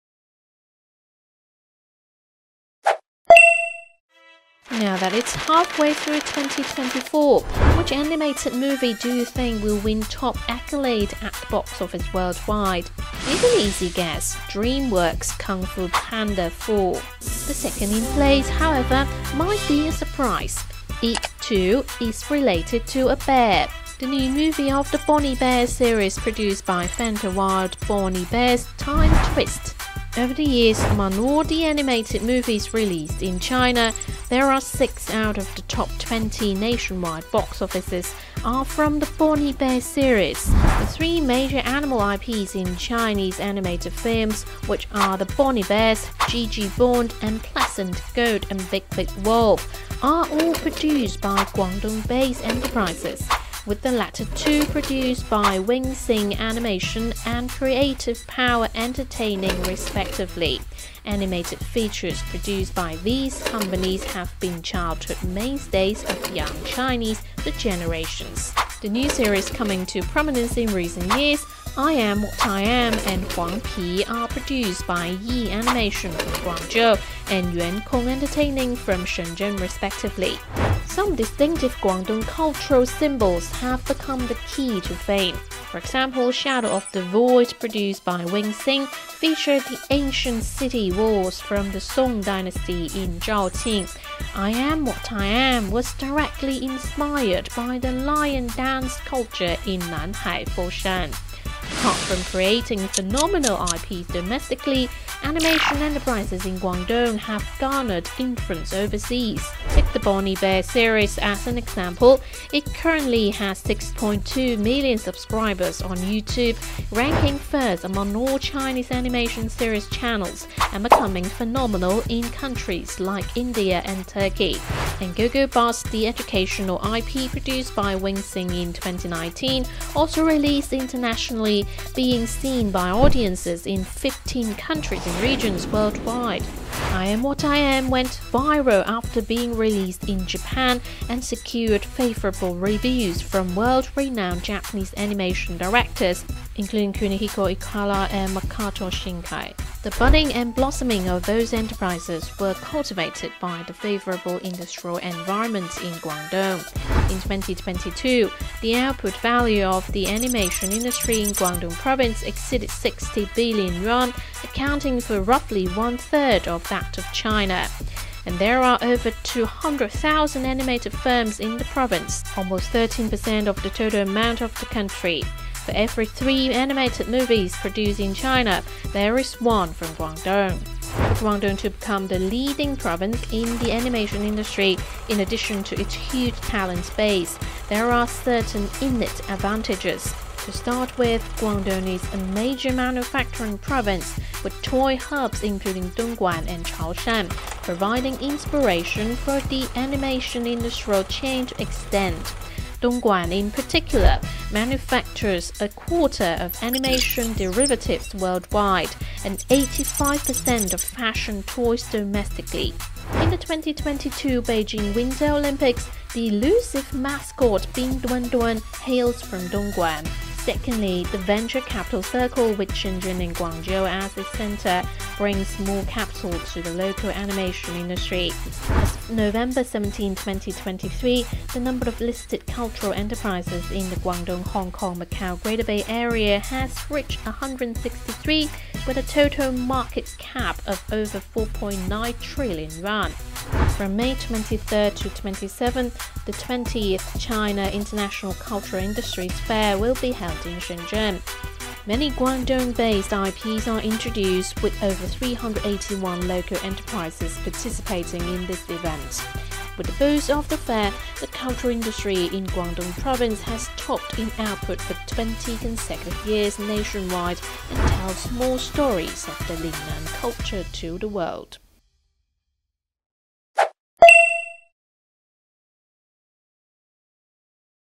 Companies in the GBA | How do Guangdong Animation Studios Ride the Wave?
The 20th China (Shenzhen) International Cultural Industries Fair kicked off on May 23, showcasing numerous animation IPs from Guangdong. From Pleasant Goat and Big Big Wolf, Boonie Bears, to I Am What I Am and Super Wings, why has Guangdong animation been able to shape the childhoods of several generations of Chinese children? Click the video to see how Guangdong animation companies are making waves.